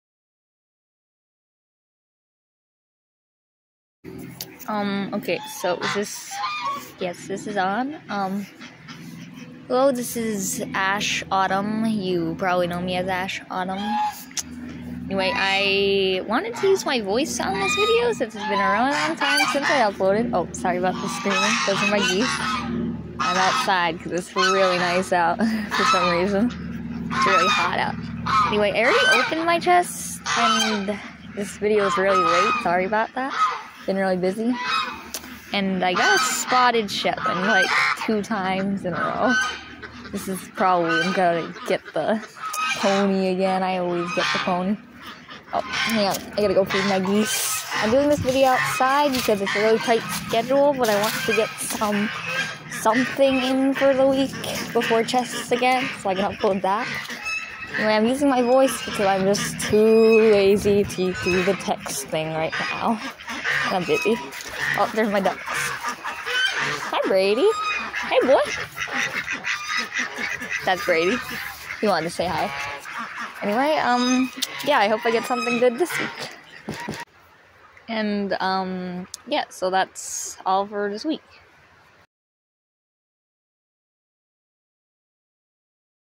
chest opening with my voice!! (already opened off camera)
sorry about the geese lol, they are happy to see me. I also have a cold and I'm outside because the weather is pretty warm. first time doing a voice over so obviously it isn't too good 😊